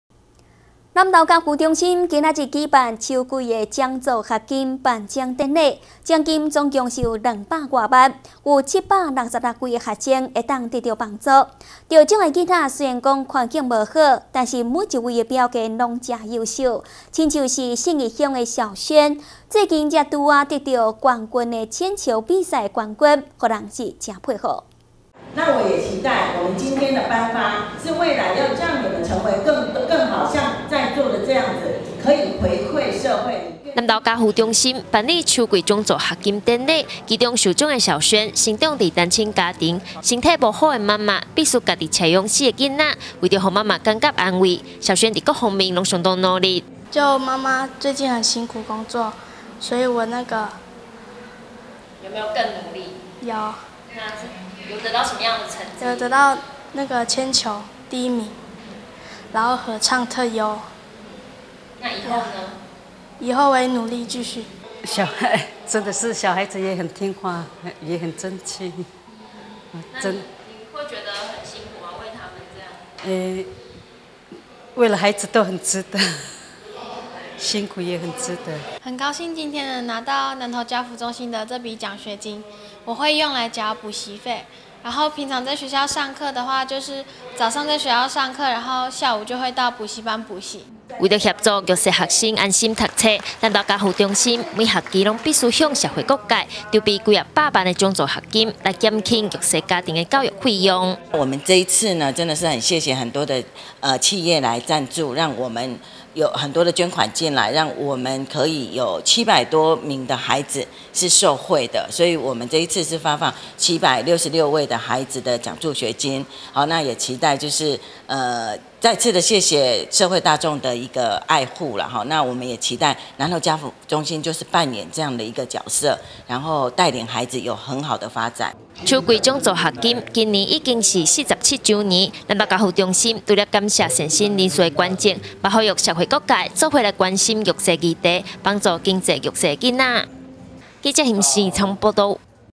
南投新聞 家扶秋季獎助學金頒發
南投家扶中心，舉辦秋季獎助學金典禮，頒發216萬2000元，總共有766位國小、國中以及高中職學生受到幫助。這些孩子都是生長在困苦家庭，卻可以有優秀表現，像來自信義的小萱，最近才剛得到縣運鉛球冠軍，在畫圖與唱歌也都很好的成績。